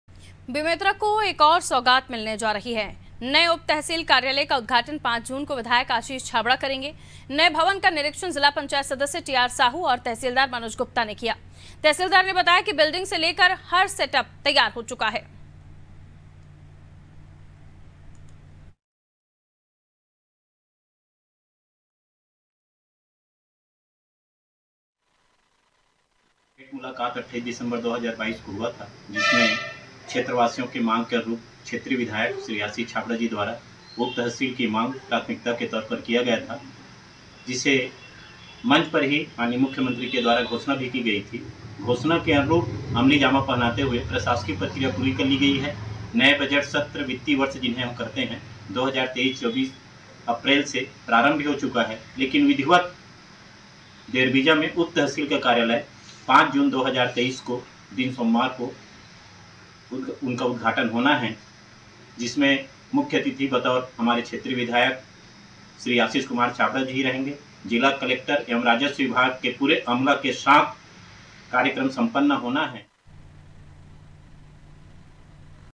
Bemetara : जिले को मिलेगी बड़ी सौगात, उपतहसील कार्यालय बनकर तैयार || Anaadi Tv
बेमेतरा को एक और सौगात मिलने जा रही है...नए उपतहसील कार्यालय का उद्घाटन 5 जून को विधायक आशीष छाबड़ा करेंगे... नए भवन का निरीक्षण जिला पंचायत सदस्य टीआर साहू और तहसीलदार मनोज गुप्ता ने किया... तहसीलदार ने बताया कि, बिल्डिंग से लेकर हर सेटअप तैयार हो चुका है...

COMING SOON
 DEN, AIRTEL